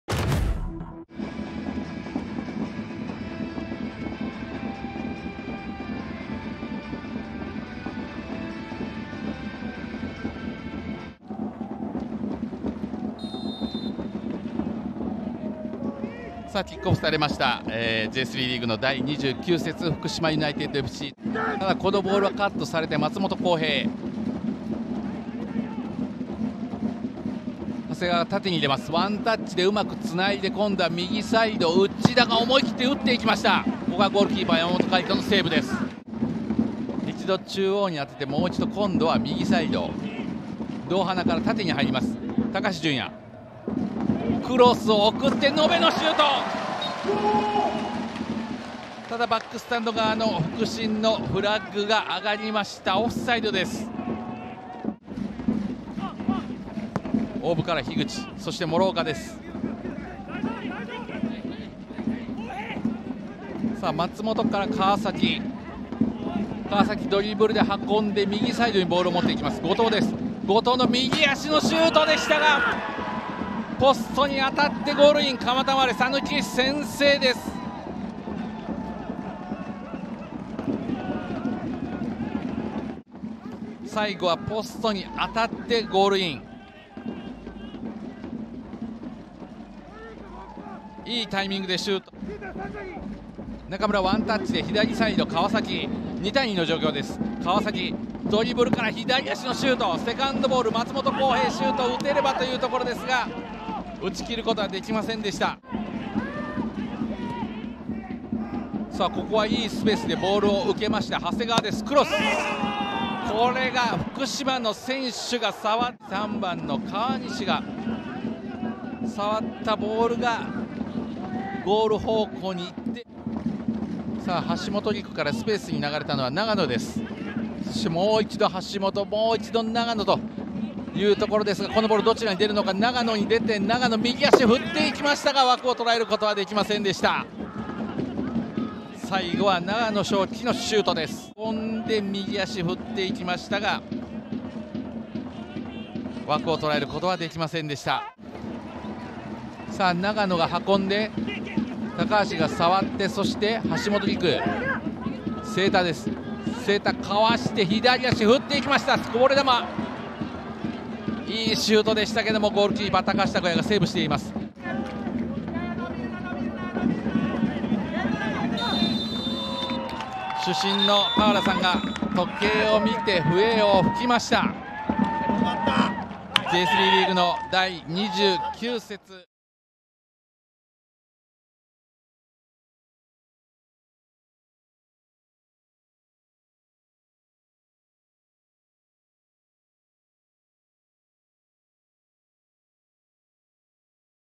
【公式】ハイライト：福島ユナイテッドＦＣvsカマタマーレ讃岐 明治安田生命Ｊ３リーグ 第29節 2022/10/16
2022年10月16日（日）に行われた明治安田生命Ｊ３リーグ 第29節 福島vs讃岐のハイライト動画です。